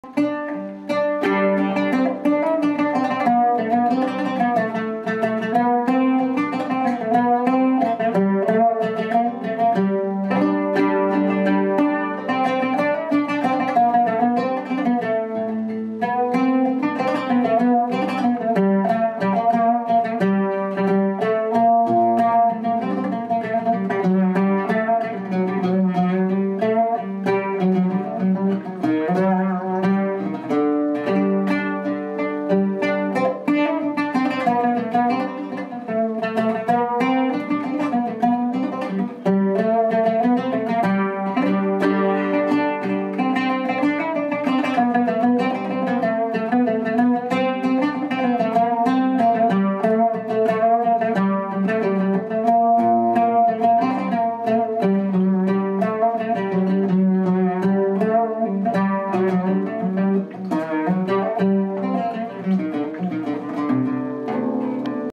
walnut oud ud
The Oud Is Made  in Izmir, Turkey
Solid Woods
Bowl: Walnut Wood
Face: Cedar
Keyboard: Wood
Pegs: Black Wood
It Comes With
Softcase
Extra Of Strings
Picks(Plectrum)
6 Courses 11 Strings
C C Strings